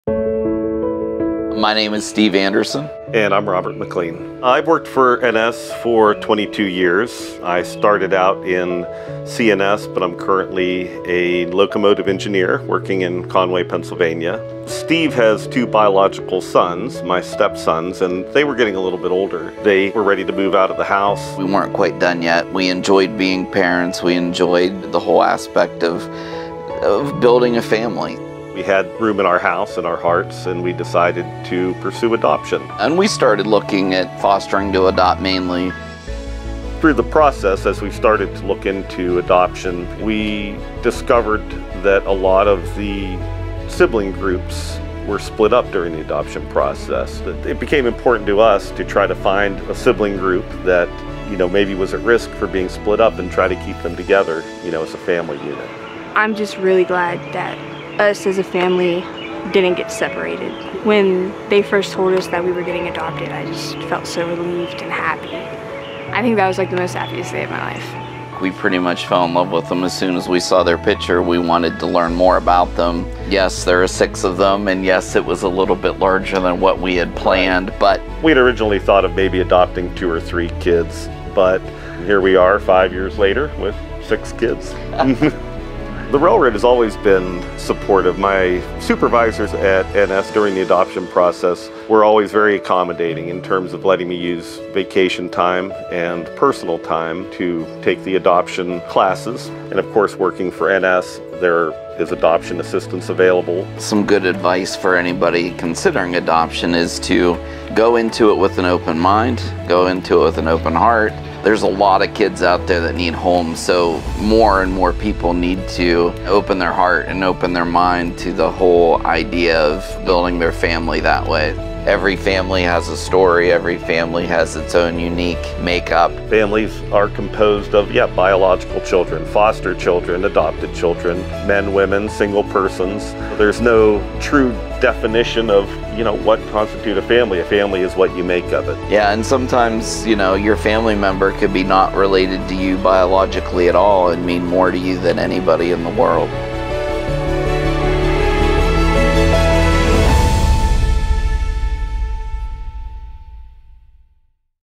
Norfolk Southern Engineer and Husband Adopt Six Siblings
This Father's Day weekend, we celebrate the incredible story of Norfolk Southern Engineer Robert Mclean and his husband, Steve Anderson, who have opened their hearts and home to six wonderful siblings through adoption. 👨‍👨‍👧‍👦

At Norfolk Southern, we recognize that families come in all shapes and sizes, and we're dedicated to supporting our employees, both at work and at home. Our family planning programs, including adoption and surrogacy assistance, support all NS families on their unique journeys.

Happy Father's Day to all the amazing dads out there!